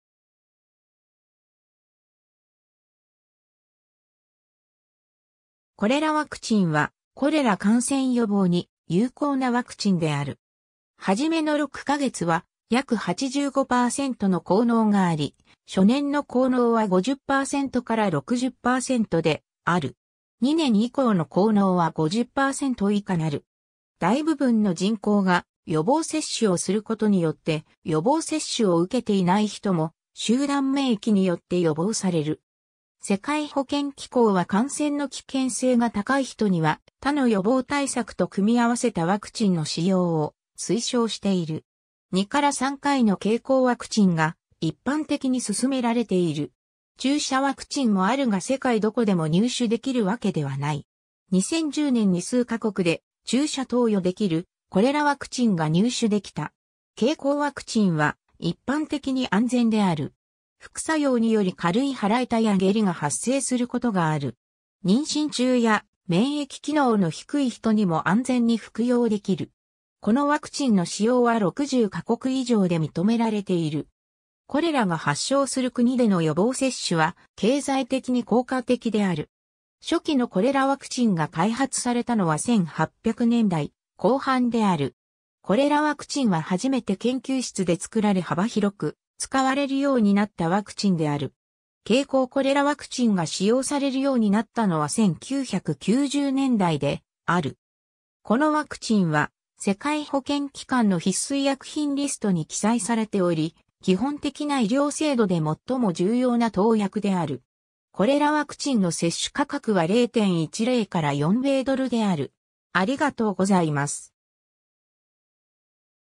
コレラワクチン
コレラワクチンはコレラ感染予防に有効なワクチンである。
初めの6か月は約85%の効能があり、初年の効能は50%～60%である。
2年以降の効能は50%以下なる。
大部分の人口が予防接種をすることによって予防接種を受けていない人も集団免疫によって予防される。
 世界保健機構は感染の危険性が高い人には他の予防対策と組み合わせたワクチンの使用を推奨している。
2～3回の経口ワクチンが一般的に勧められている。
注射ワクチンもあるが世界どこでも入手できるわけではない。
2010年に数カ国で注射投与できるコレラワクチンが入手できた。
 経口ワクチンは一般的に安全である。
副作用により軽い腹痛や下痢が発生することがある。
 妊娠中や免疫機能の低い人にも安全に服用できる。
このワクチンの使用は60か国以上で認められている。
コレラが発症する国での予防接種は経済的に効果的である。
 初期のコレラワクチンが開発されたのは1800年代後半である。
コレラワクチンは初めて研究室で作られ幅広く使われるようになったワクチンである。
経口コレラワクチンが使用されるようになったのは1990年代である。
このワクチンは世界保健機関の必須医薬品リストに記載されており、基本的な医療制度で最も重要な投薬である。
コレラワクチンの接種価格は0.10から4.0米ドルである。